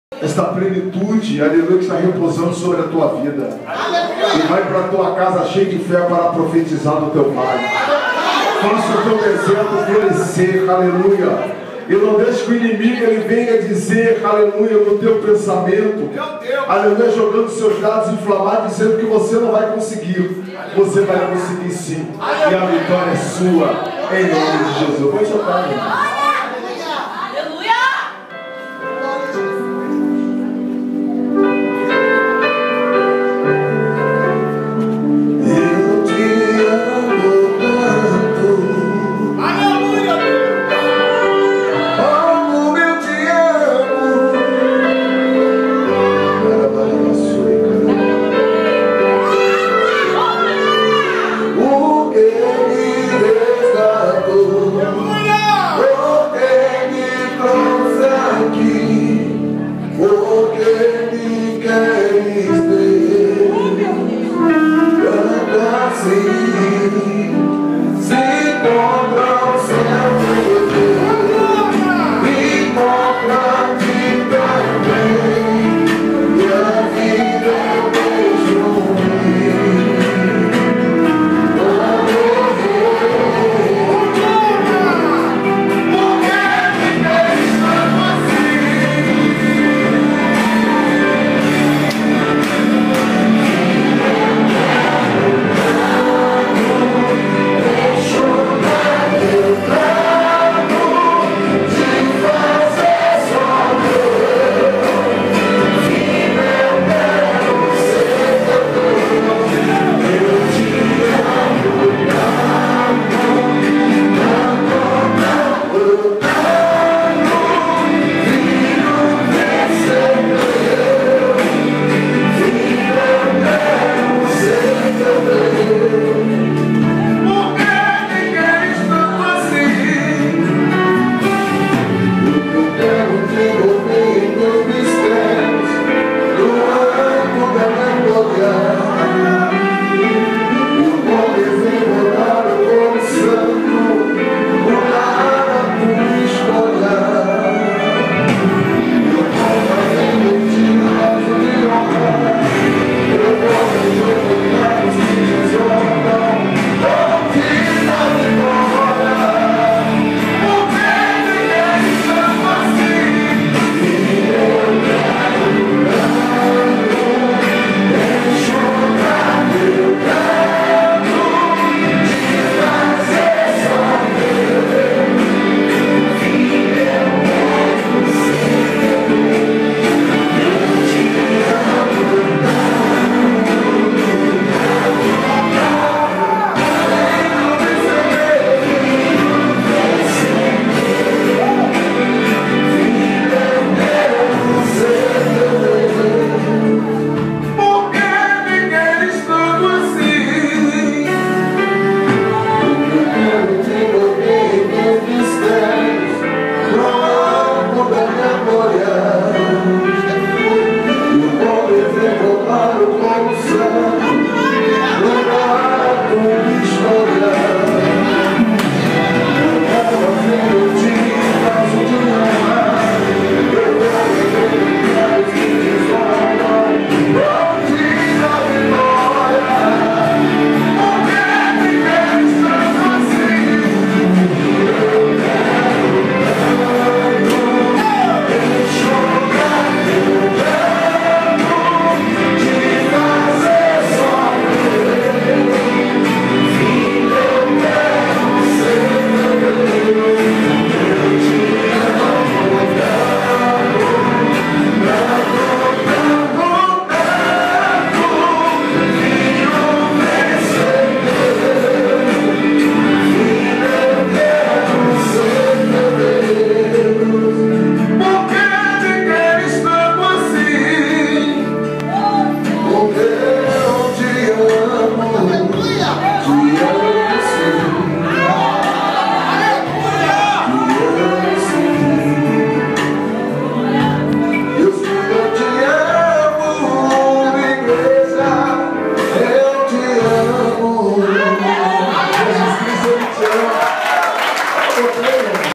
Igreja Pentecostal Resgatando Soldados Feridos - Aldecir Carvalho
Igreja Pentecostal Resgatando Soldados Feridos
Festividade 7 anos da igreja - Perfeição!
Tema: SALMOS 91!
| Pastora Shirley.
Direção: Missionária Elaine, Obreira Natália.